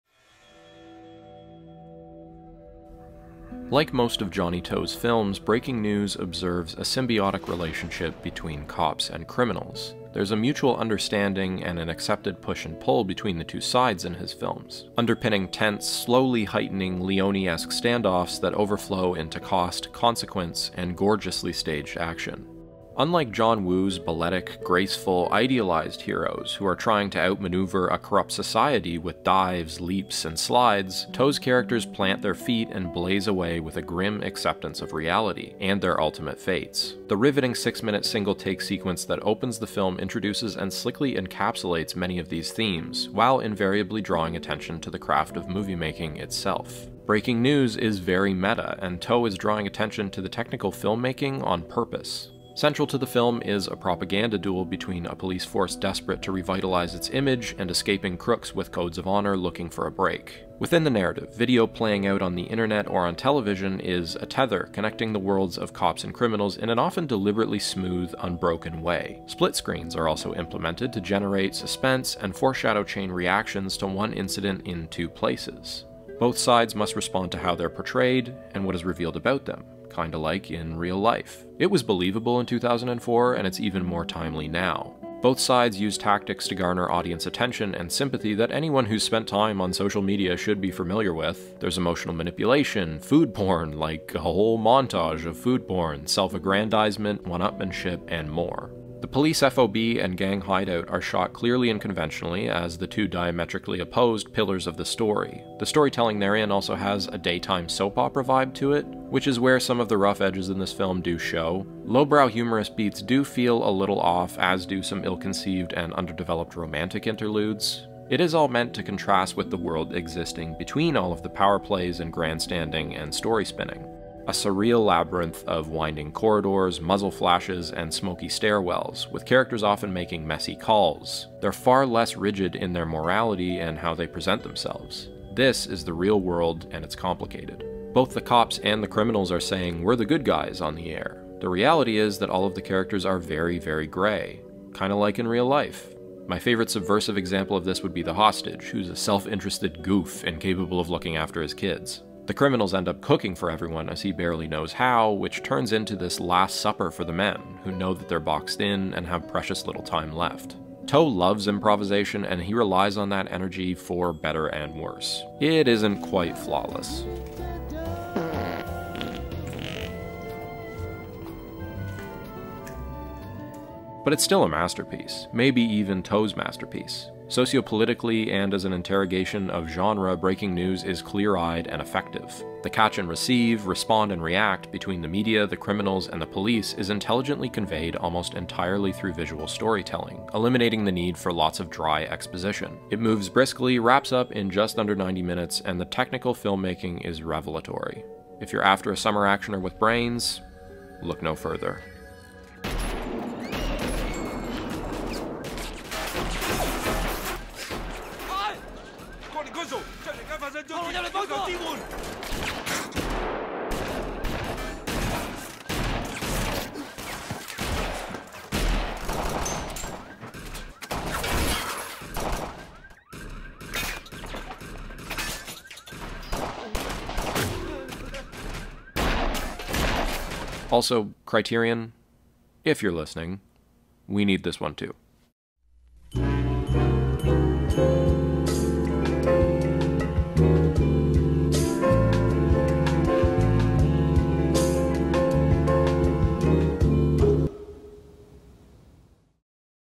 A Propaganda Duel: Johnnie To's Breaking News
Johnnie To's "Breaking News" works as a heroic-bloodshed-style action flick. However, the meta-textual propaganda war between crooks with cellphones, and cops partnered with TV agencies, is where it truly shines.